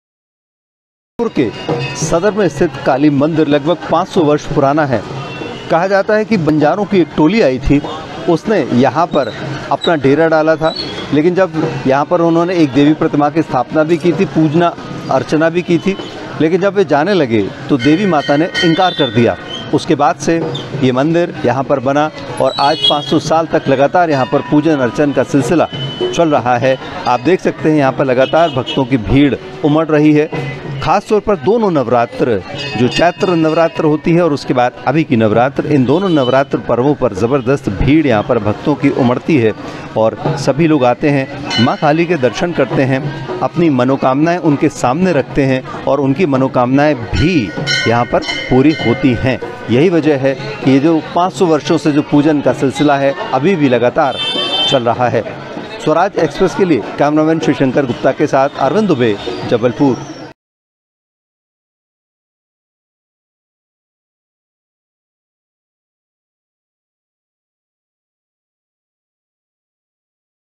Navratra Parva Jabalpur:संस्कारधानी के दुर्गा मंदिरों में की इन पुराने भव्य पंडाल के जरूर करें दर्शन
Mp Swaraj में आप देखेंगे, मध्यप्रदेश की हर जरूरी ख़बर।

News
Latest News - Breaking News and Top Video News